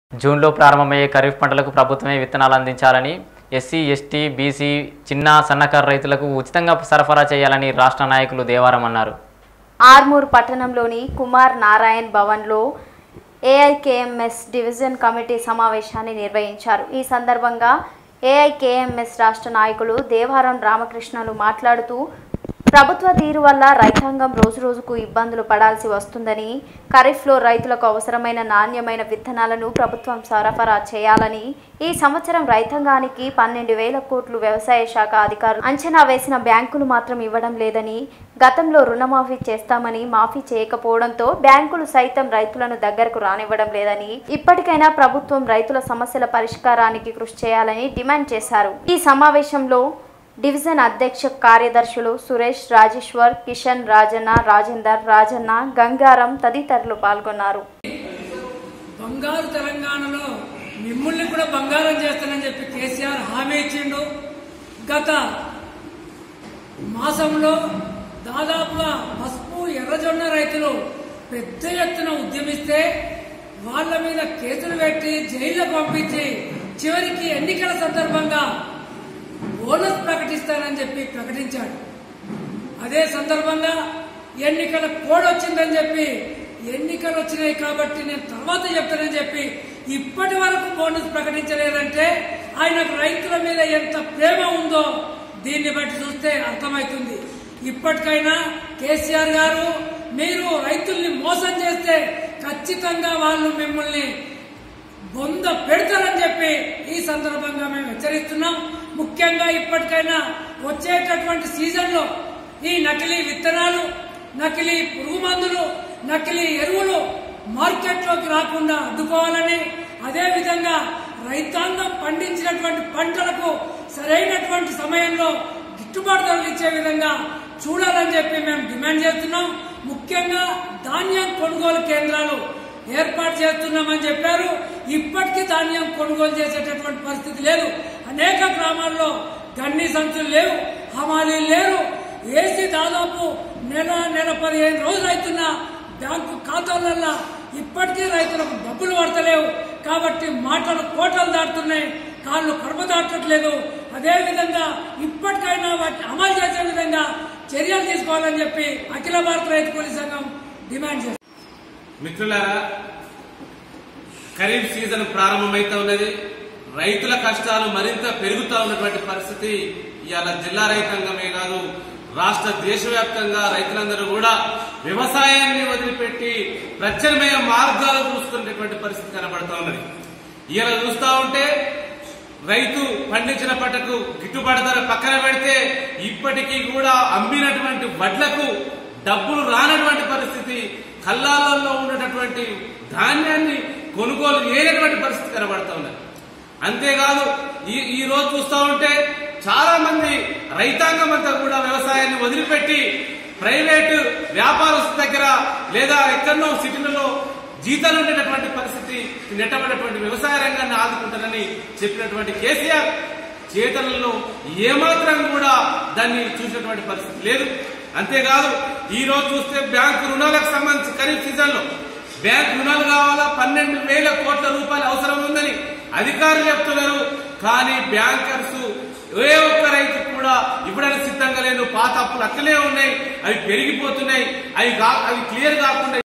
TS24 News జూన్ లో ప్రారంభమయ్యే ఖరీఫ్ పంటకు ప్రభుత్వం సకాలంలో విత్తనాలు అందించాలి
లో ప్రారంభమయ్యే ఖరీఫ్ పంటకు ప్రభుత్వం సకాలంలో విత్తనాలు అందించాలి